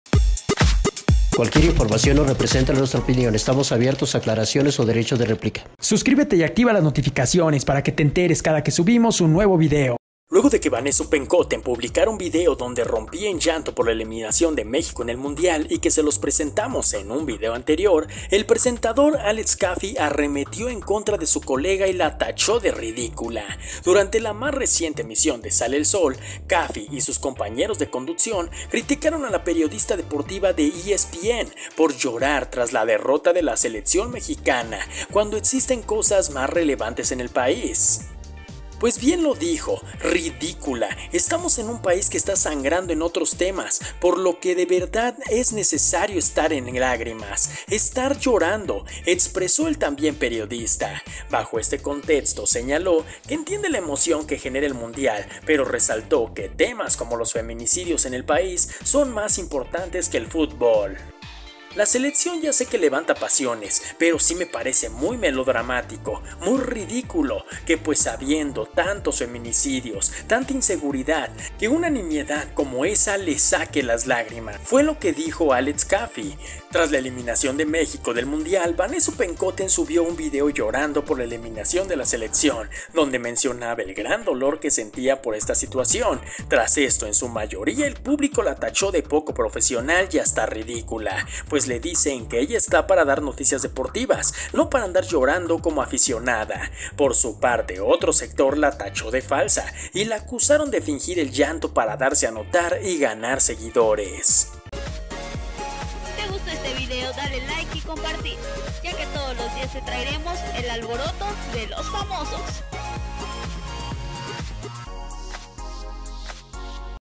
Alex Kaffie llama ridicuIa a periodista de ESPN ex de Televisa por llorar por la Selección Mexicana
No te pierdas diariamente las mejores noticias de tus artistas favoritos solo aqui, en el ALBOROTO DE LOS FAMOSOS